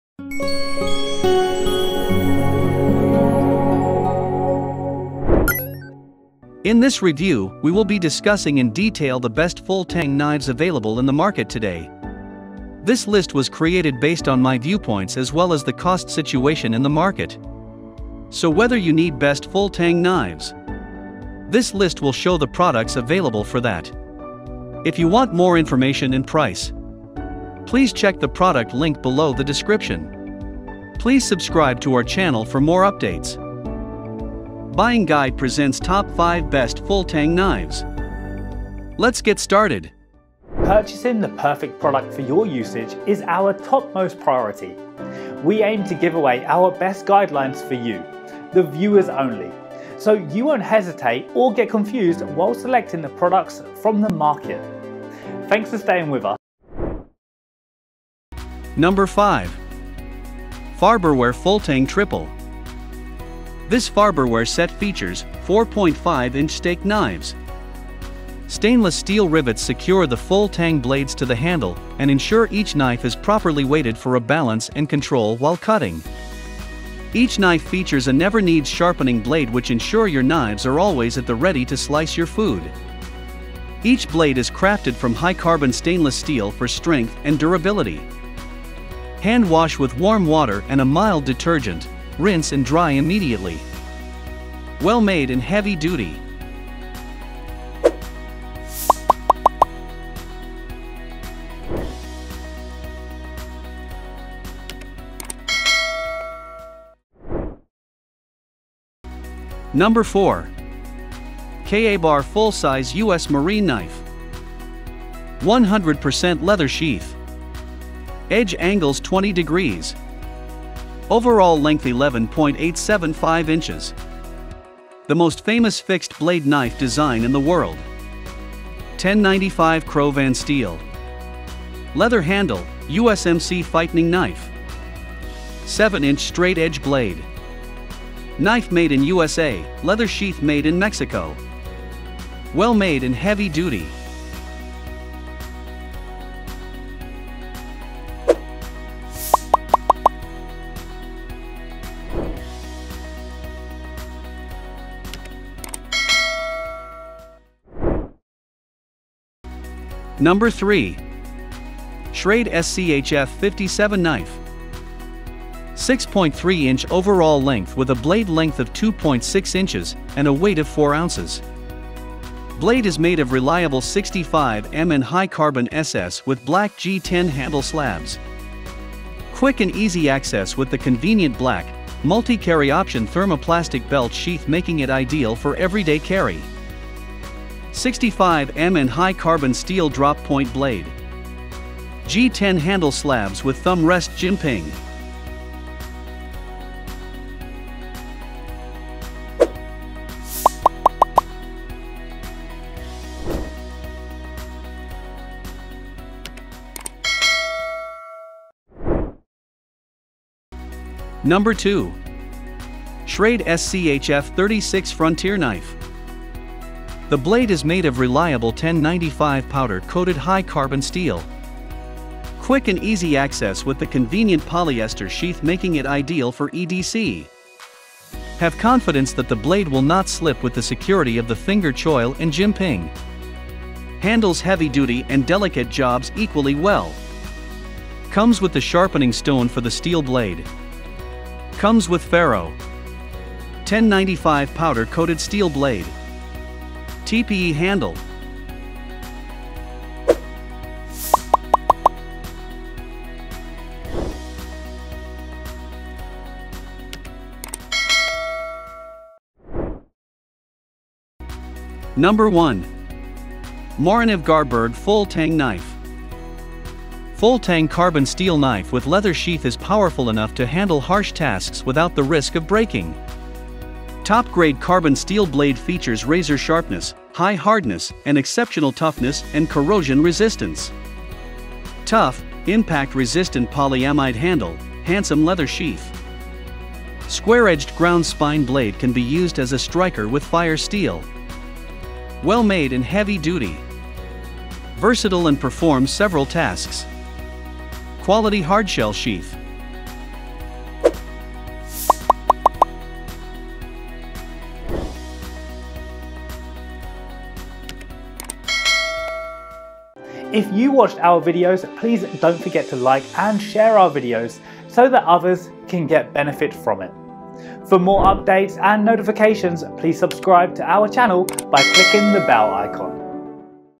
Best Full Tang Knives in 2022 - Reviewed and Top Rated!
In this video, you’ll know about the exciting features of Full Tang Knives at the best price. So, we’ve chosen the Best Full Tang Knives based on quality, features, performance, and price. Choose any of them that matches your considerations.

Best Sellers in 2022
00:00 - Introduction : Best Full Tang Knives in 2022

00:58 - 5. Farberware Full-Tang Triple Rivet Knife

01:49 - 4. KA-BAR Full Size US Marine Knife

02:40 - 3. Schrade SCHF57 Knife

03:31 - 2. Schrade SCHF36 Frontier Knife

04:21 - 1. Morakniv Garberg Full Tang Knife

Best Full Tang Knives in 2022,Top Full Tang Knives in 2022,Best Full Tang Knives Reviews